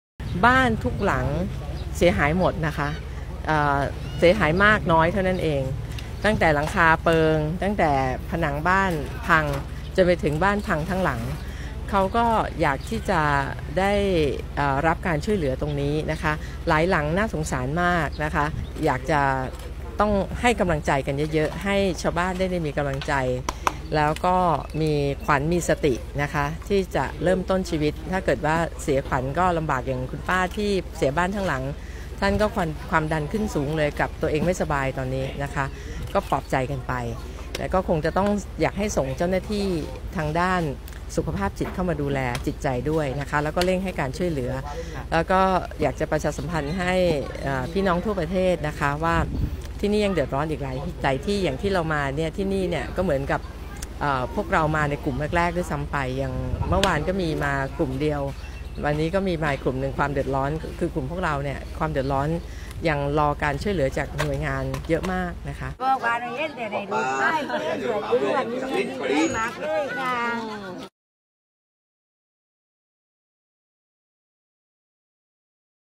" สุดารัตน์ เกยุราพันธุ์ " ลงพื้นที่ ให้กำลังใจผู้ประสบภัย " พายุปาบึก " นครศรีธรรมราช
" สุดารัตน์ เกยุราพันธุ์ " ลงพื้นที่ ให้กำลังใจผู้ประสบภัย " พายุปาบึก " นครศรีธรรมราช เพื่อไทย Jan 7, 2019